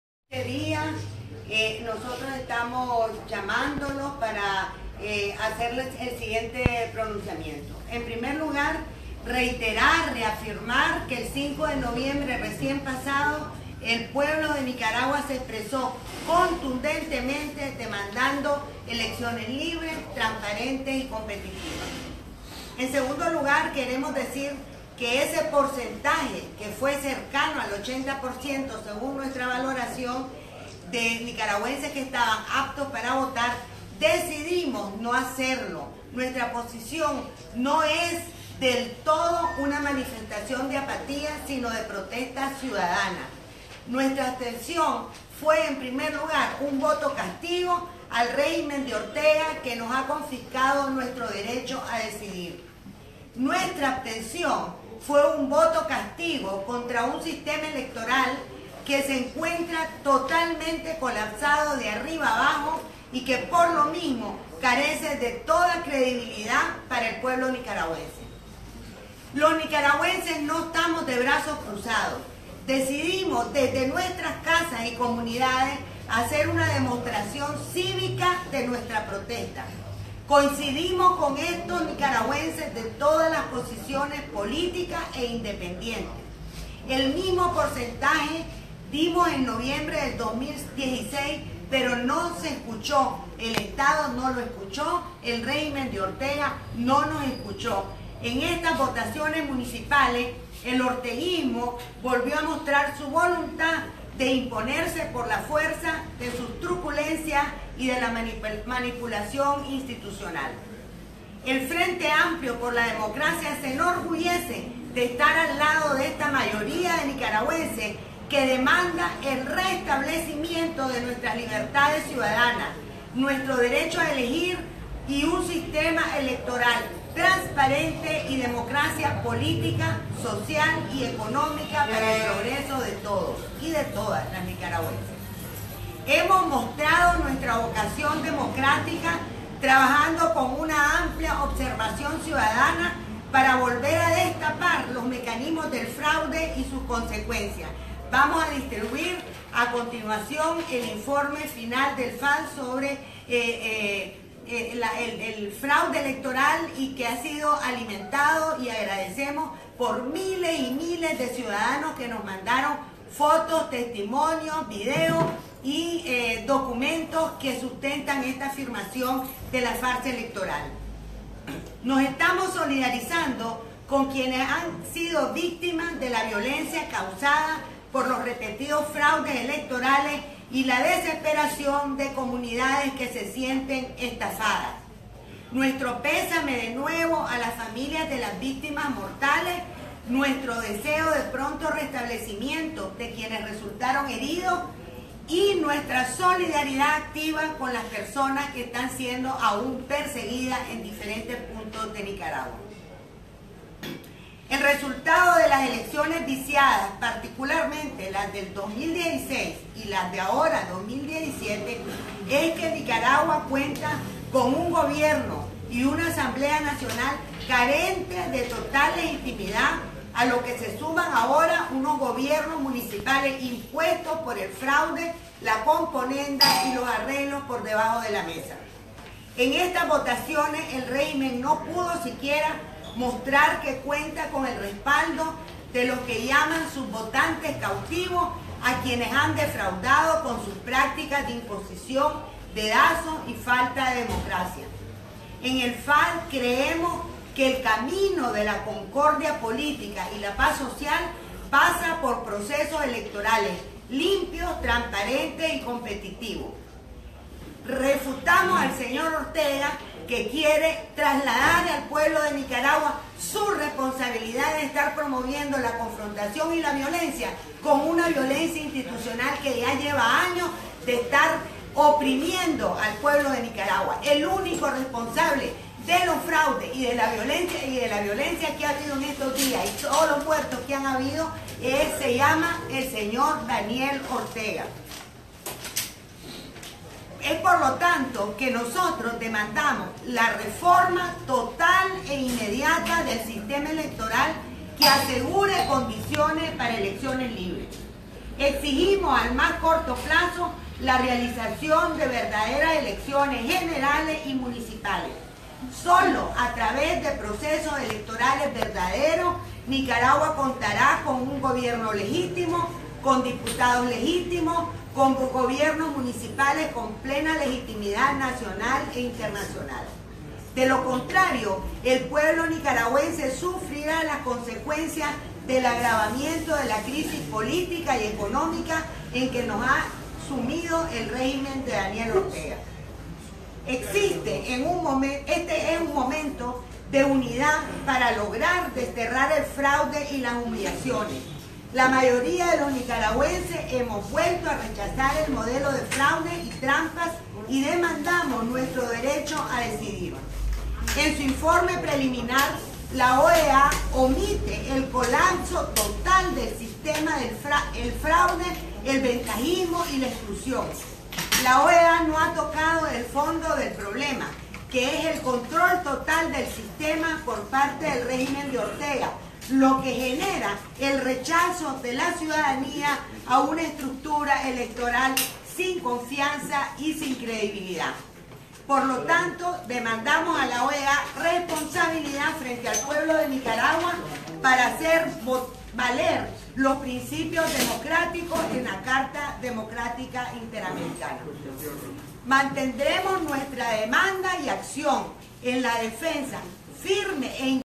FAD demanda nuevas elecciones generales
Posición del FAD respecto a las votaciones municipales.

Este es un video del canal MRS Tv